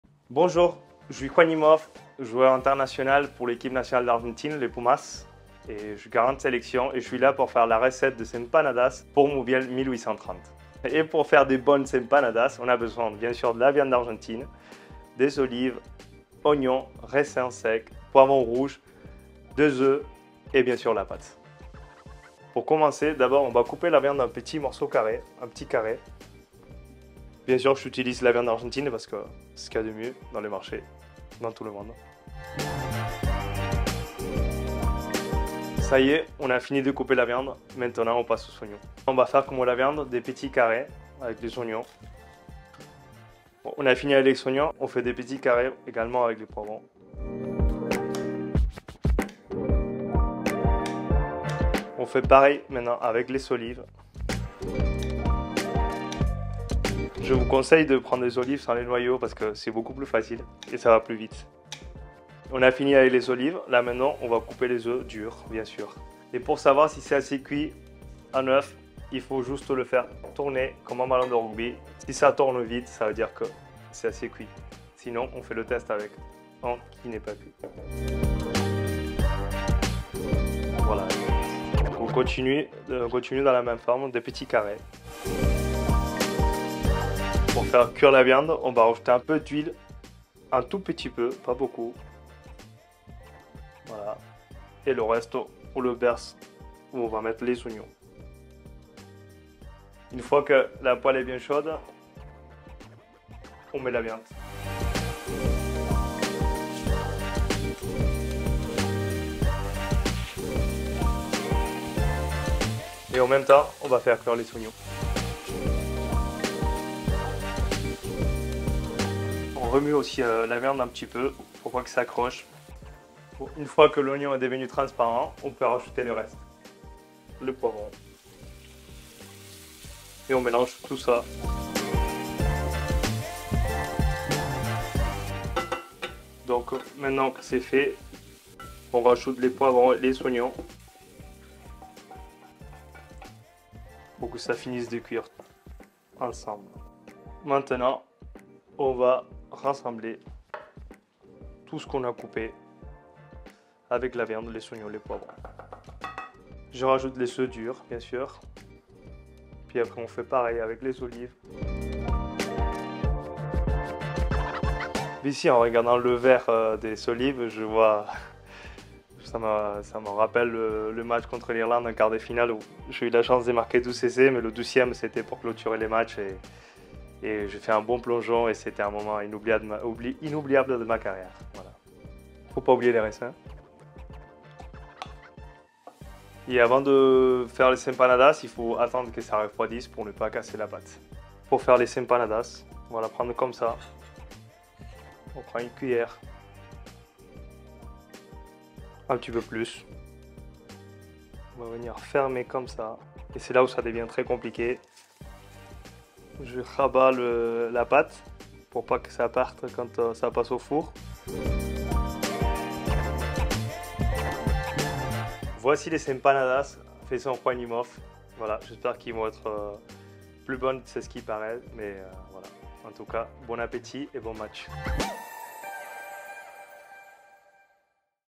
La Recette de Juan Imhoff
JUAN IMHOFF - 40 SELECTIONS EN EQUIPE NATIONALE D’ARGENTINE

Joueur des Pumas, équipe nationale d'Argentine, Juan Imhoff honore sa première cape internationale avec l'équipe d'Argentine le 20 mai 2009 contre l'équipe du Chili.

Juan Imhoff est aujourd’hui titulaire indiscutable à l'aile du Racing 92 et s’affiche comme le meilleur marqueur d'essais de son club depuis l'ère professionnelle. Cela le place dans les meilleurs marqueurs de l'histoire aussi bien en Top 14 qu'en Coupe d'Europe.

Et certainement le meilleur chef d’empanadas argentins de l’hexagone ! Il enfile le tablier pour Mauviel1830 et vous concocte une spécialité de sa terre d’origine.

Yum yum !